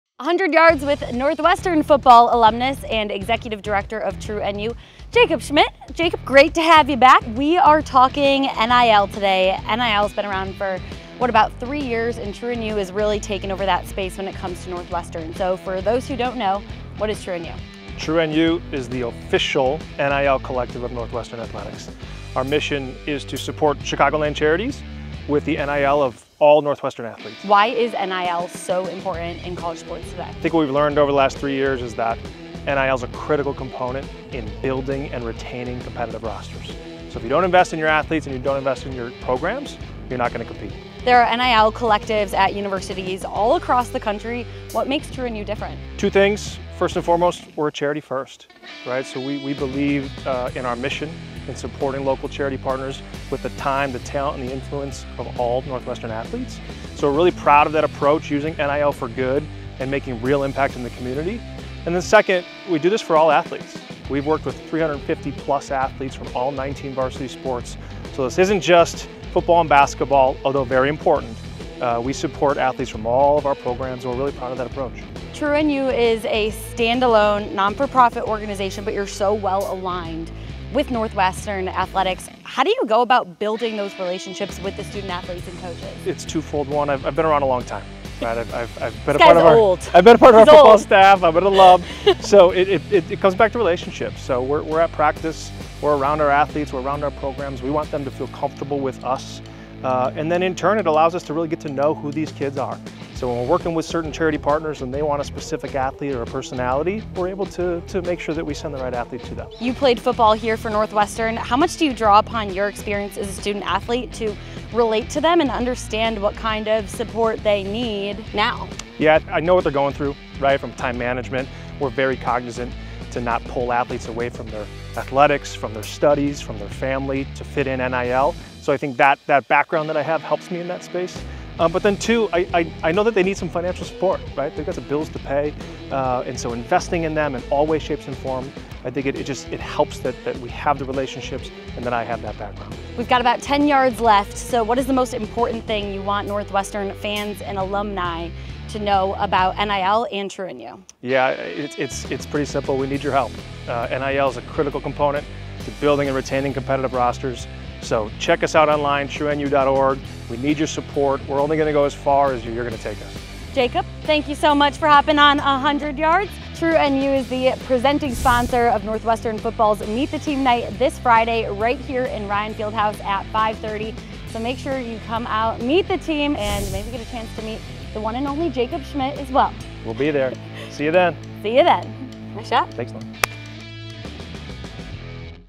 Football - 100 Yards: TrueNU Executive Director Jacob Schmidt
Join team reporter Lauren Withrow for this 100 yard walk with Jacob Schmidt, Northwestern Football alumnus and executive director of TrueNU, the official NIL partner of Northwestern Athletics.